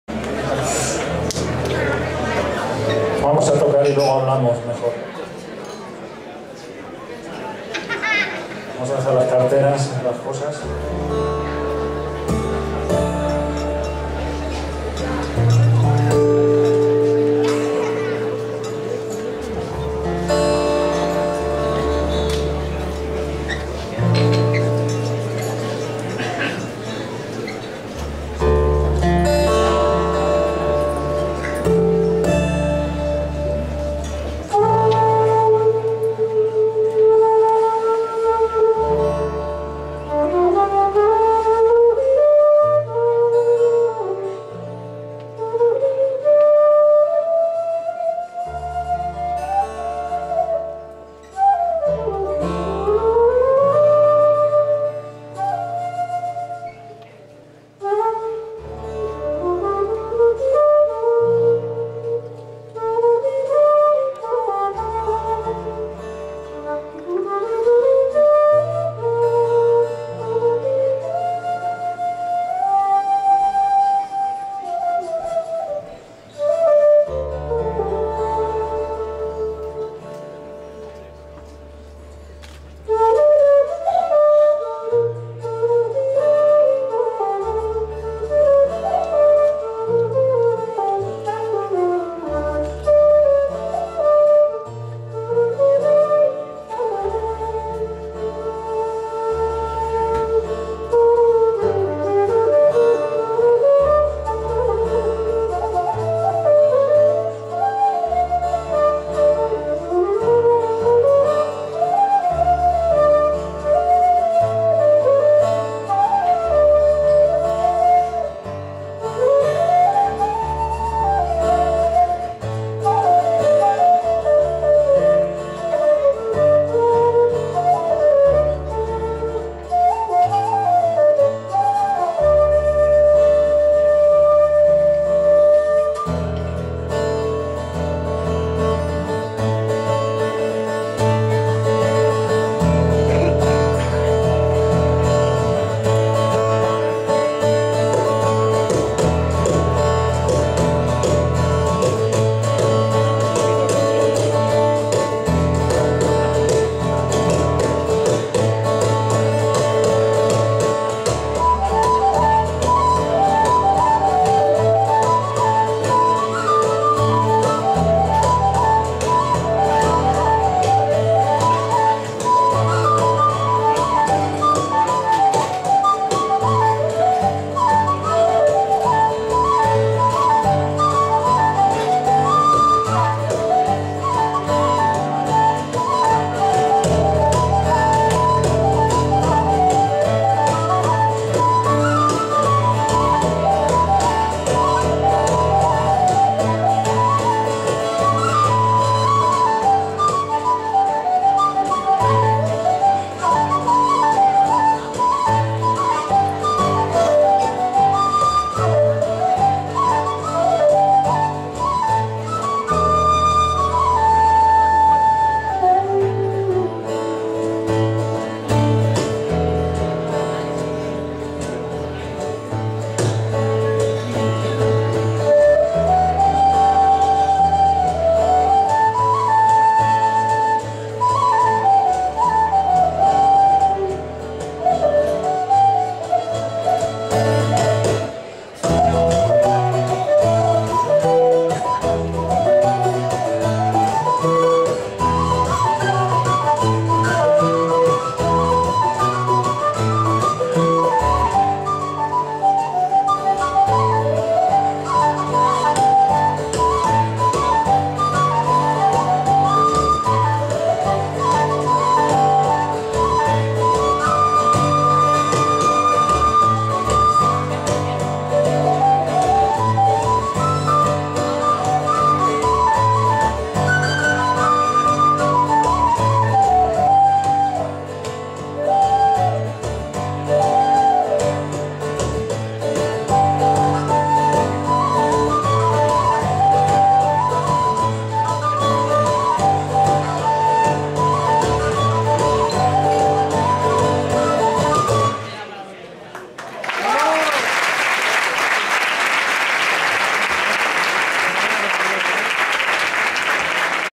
M2U03467. La Musgaña.
Festival, 20 Aniversario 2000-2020 Folklore Plaza Castilla, celebrado en la Sala Galileo el 7 de marzo de 2020. La Musgaña.